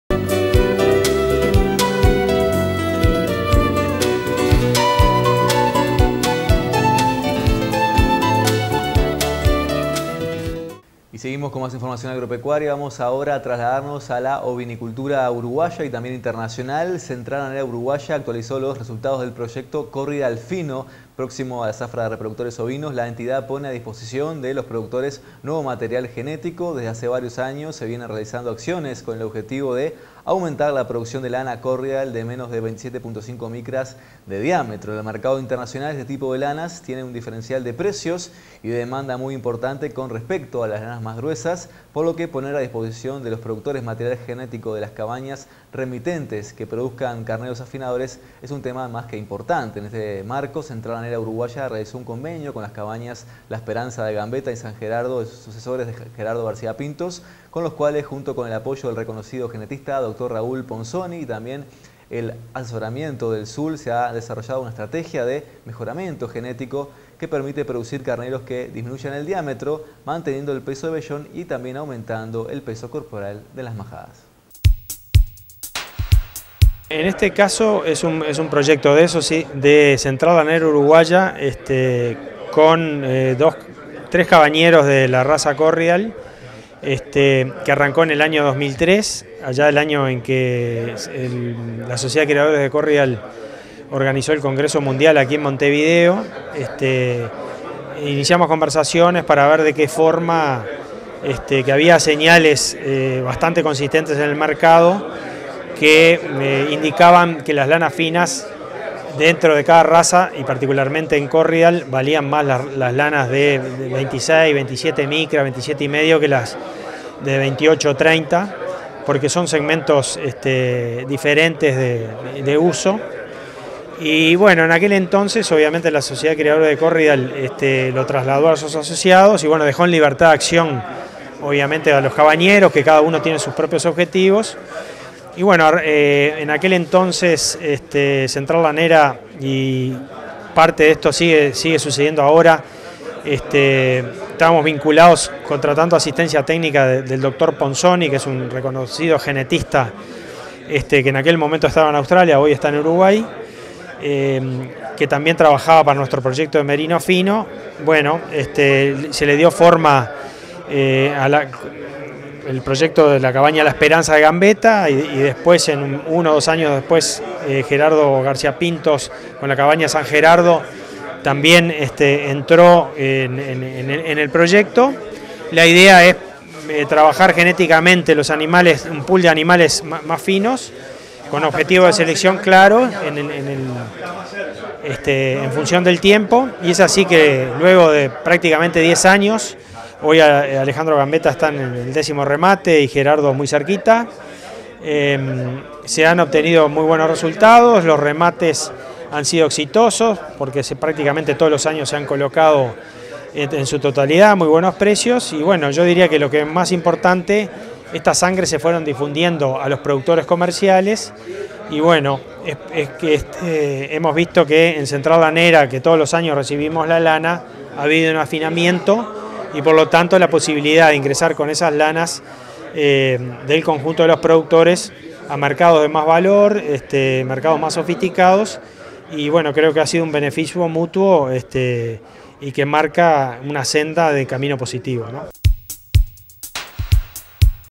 ALEJANDRO NIN
A PARTIR DEL PROYECTO CORRIEDALE FINO, CENTRAL LANERA PONE A DISPOSICION DE LOS PRODUCTORES UN NUEVO MATERIAL GENETICO PARA LA PRODUCCION DE CARNEROS AFINADORES.